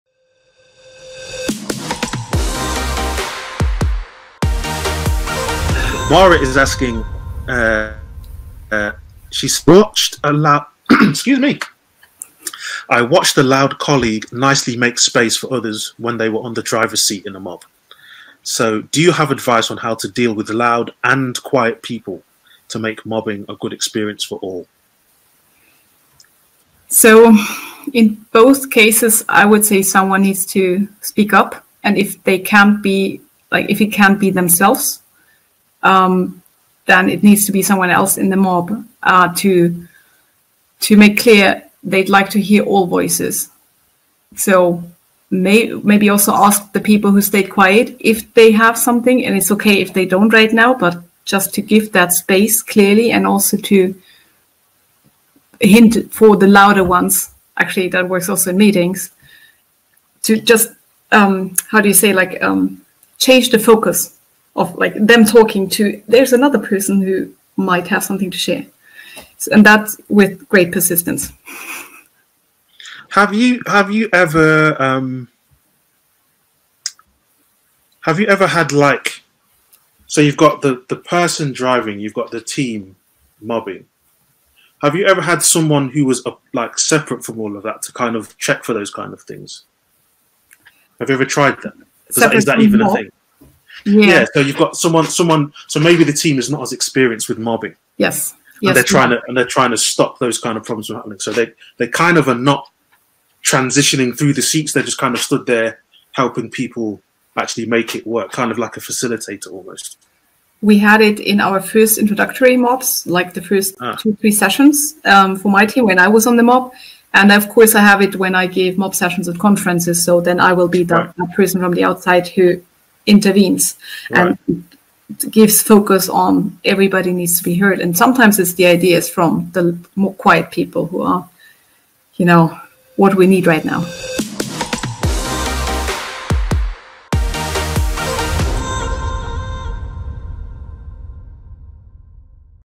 Make mobbing a good experience for both loud and quiet people | Elisabeth Hocke
Part of our Ask Me Anything series on Collaboration Pairing Mobbing with Elisabeth Hocke, hosted by Vernon Richards.

Join our Pro membership for access to TestBash content, online courses and more